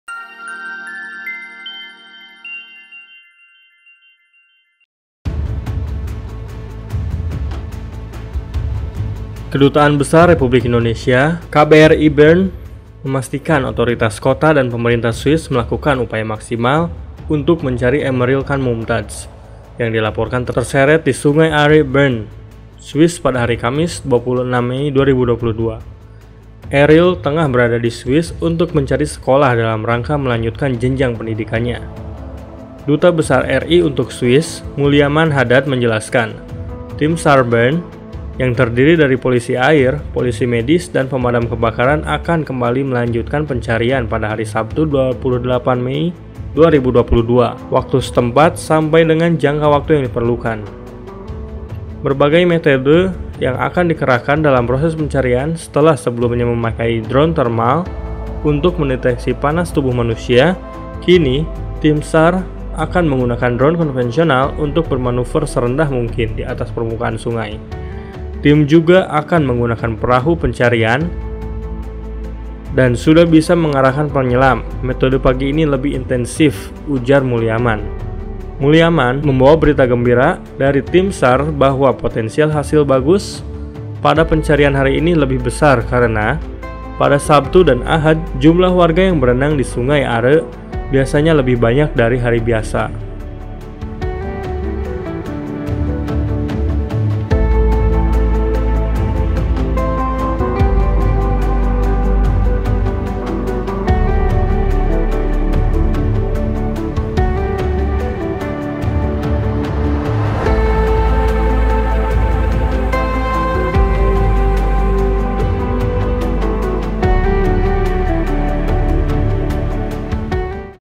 KBRI Bern Bawa Kabar Baik Soal Pencarian Eril Anak Ridwan Kamil
Kedutaan Besar Republik Indonesia (KBRI) Bern memastikan otoritas kota dan pemerintah Swiss melakukan upaya maksimal untuk mencari Emmeril Khan Mumtadz yang dilaporkan terseret di Sungai Aare Ben, Swiss pada Kamis (26/5/2022).

Eril tengah berada di Swiss untuk mencari sekolah dalam rangka melanjutkan jenjang pendidikannya.

Duta Besar RI untuk Swiss Muliaman Hadad menjelaskan, tim SAR Bern yang terdiri dari polisi air, polisi medis, dan pemadam kebakaran akan kembali melanjutkan pencarian pada Sabtu (28/5/2022) mulai pukul 08.30 waktu setempat sampai dengan jangka waktu yang diperlukan.